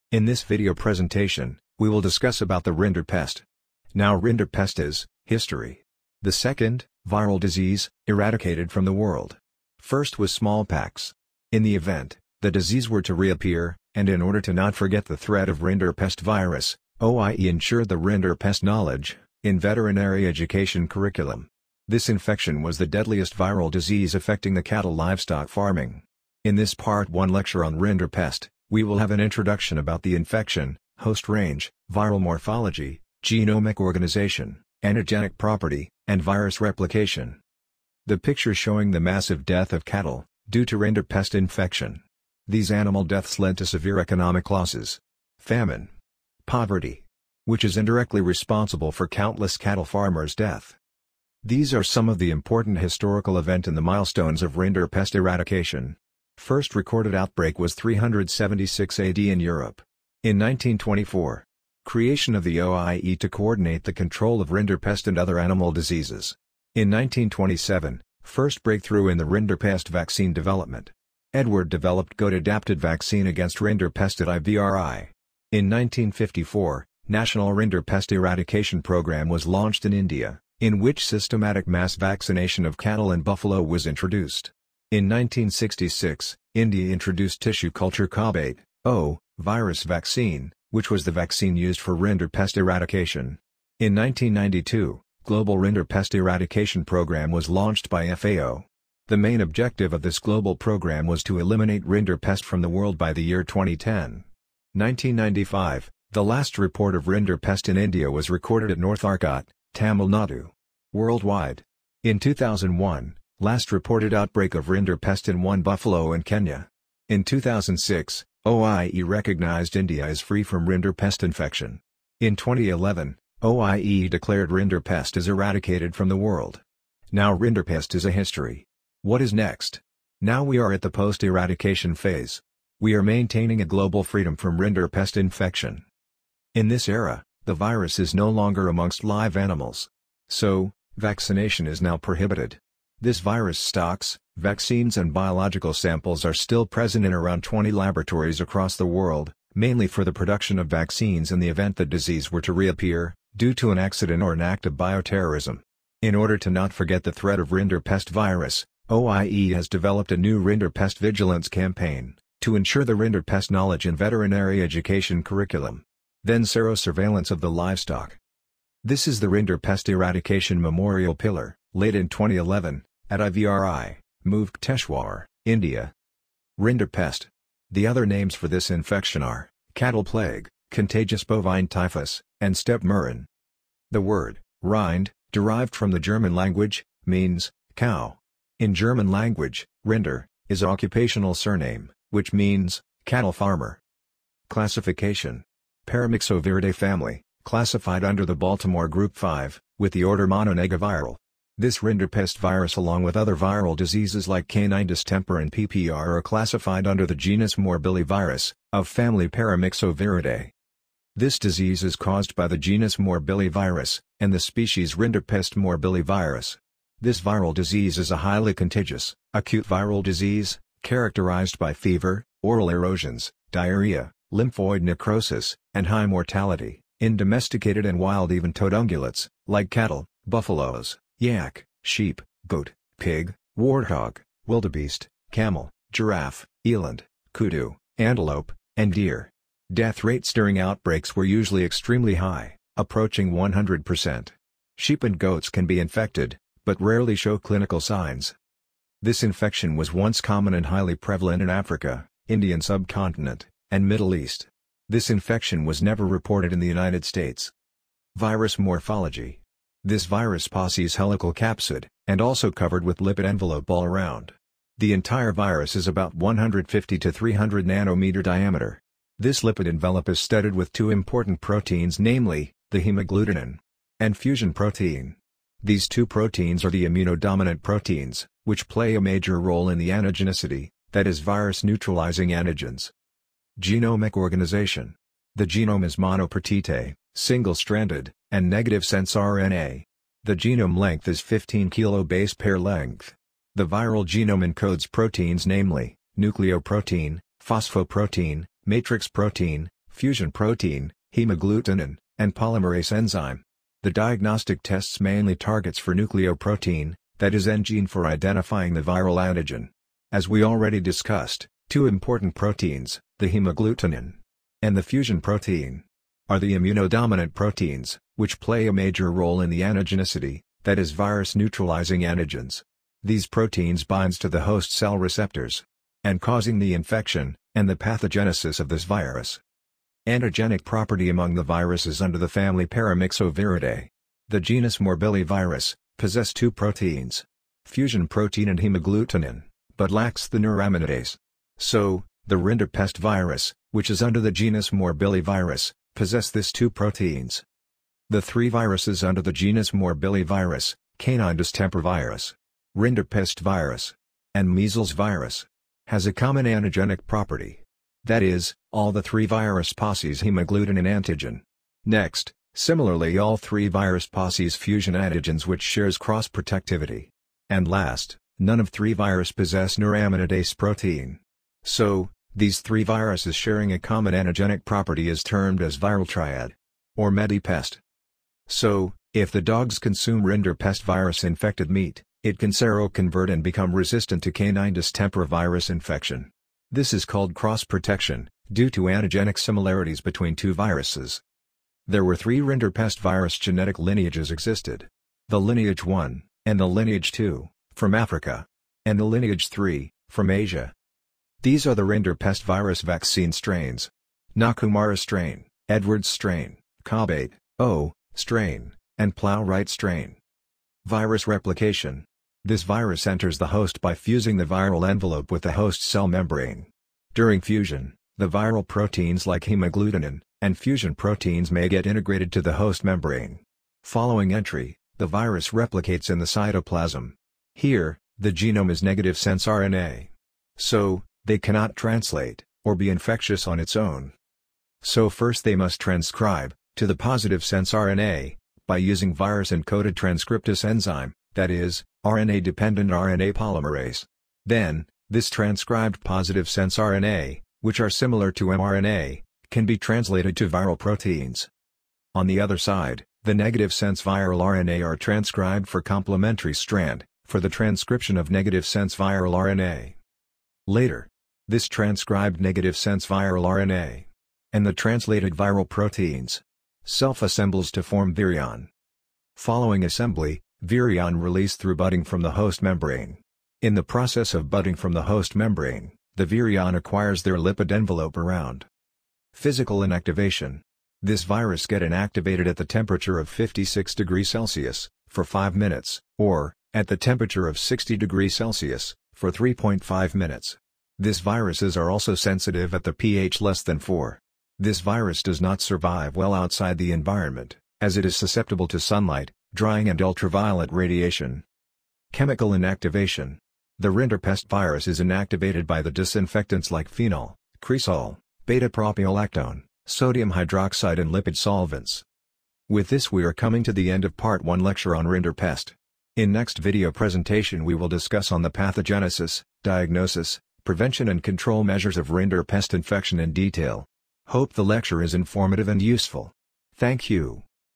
RINDERPEST | Part 1 | Microbiology | Vivek Srinivas
This video presentation describes about the Rinderpest infection, Etiology, Host range, Viral morphology, Genomic organization, Antigenic property, Virus replication, and virus physical-chemical property.

Lectures on Microbiology & Microbial Biotechnology
Dr. V. M. Vivek Srinivas, PhD